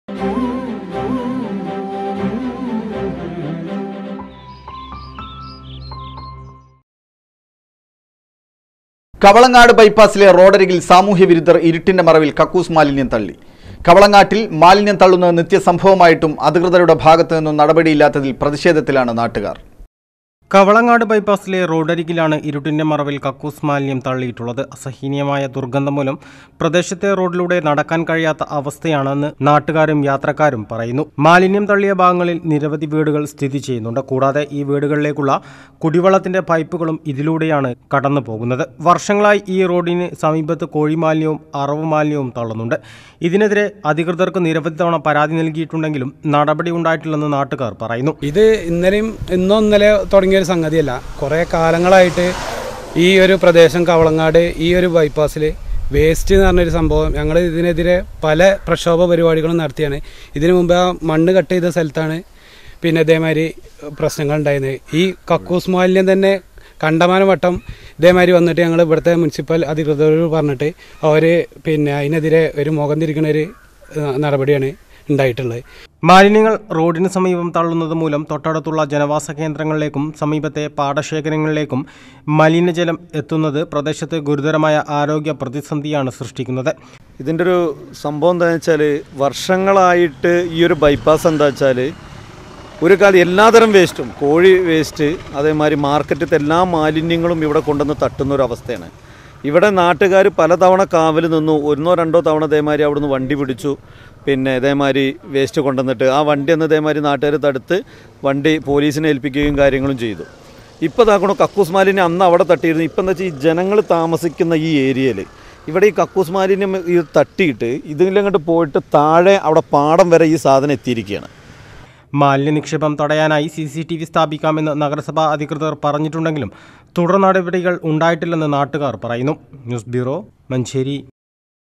SAHYANEWS
കവളങ്ങാട്   ബൈപാസിലെ റോഡിരകില്‍  സാമൂഹ്യ വിരുദ്ധര്‍ ഇരുട്ടിന്റെ മറവില്‍ കക്കൂസ് മാലിന്യം തള്ളി.